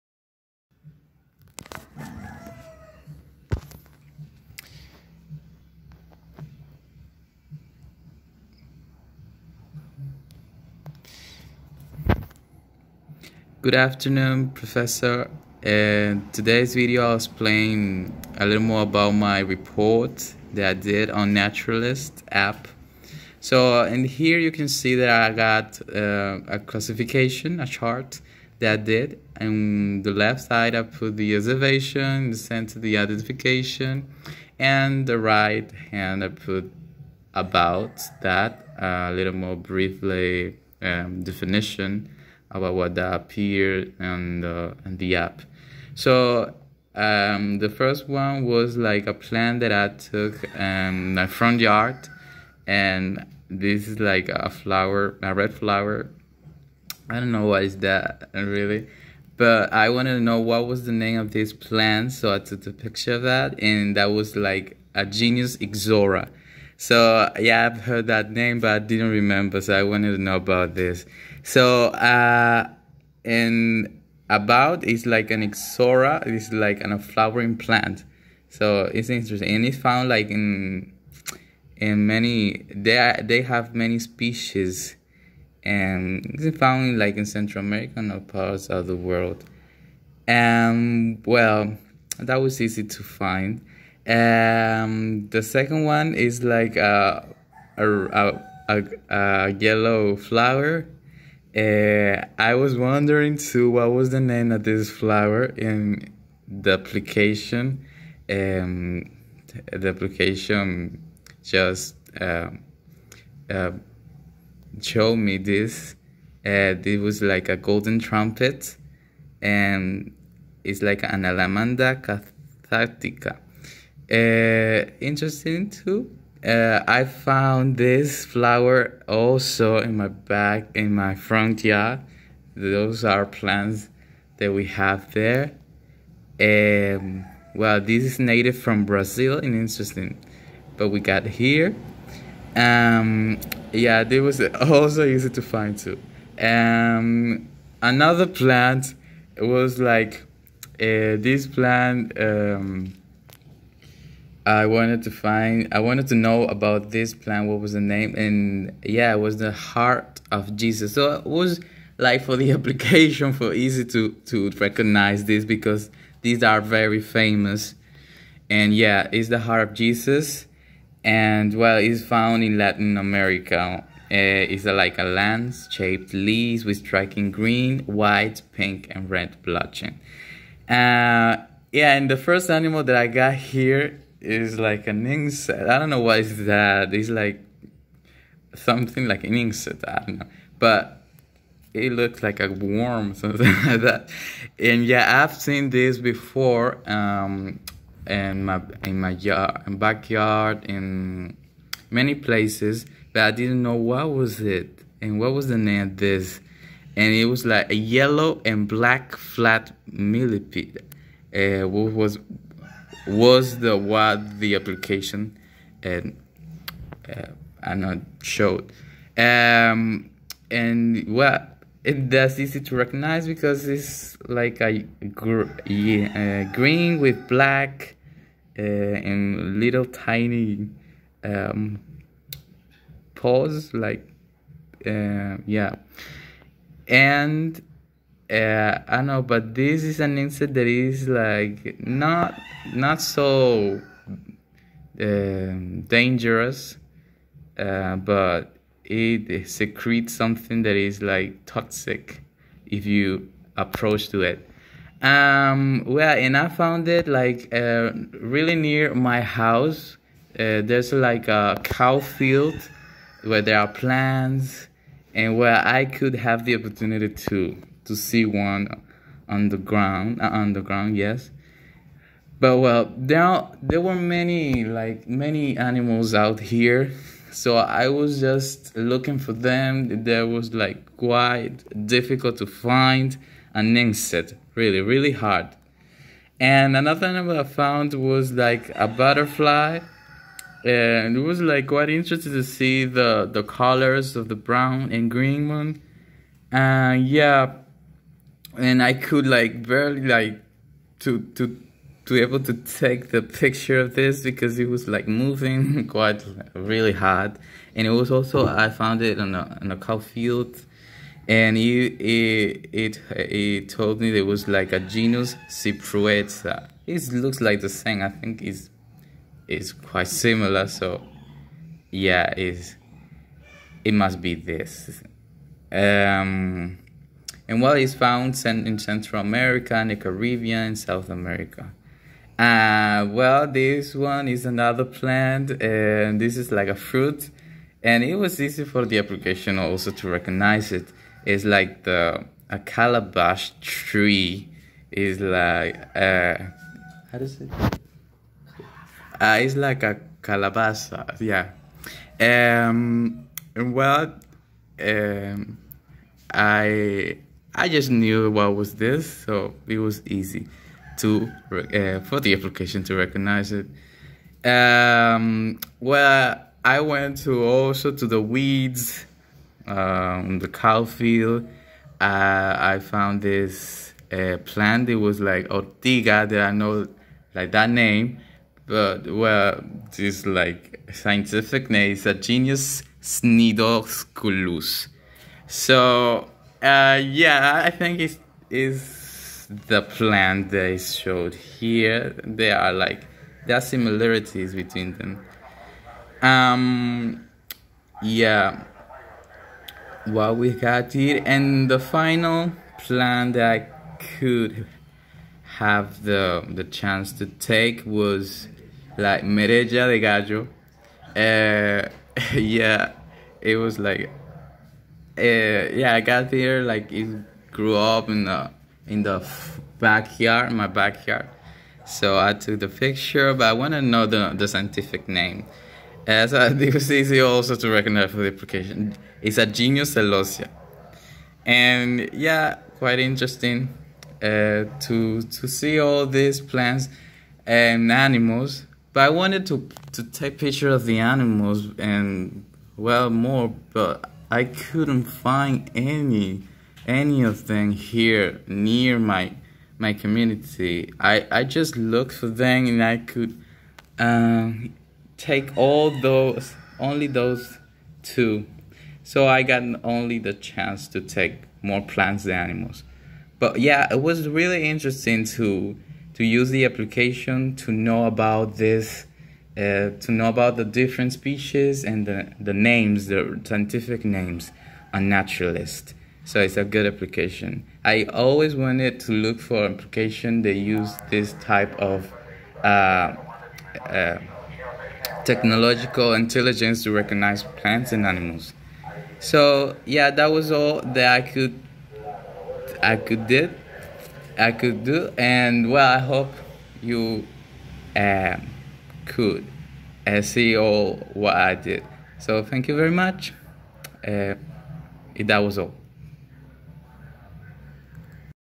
Comunicación Ejecutiva Bilingue/ Ecology: Naturalist App-my observations
It's amazing that you can learn about plants and animals in your area by just taking pictures and uploading them to the Naturalist app to know and learn about a specie you might be interested in. So, what are you waiting for? Have fun and picture the nature !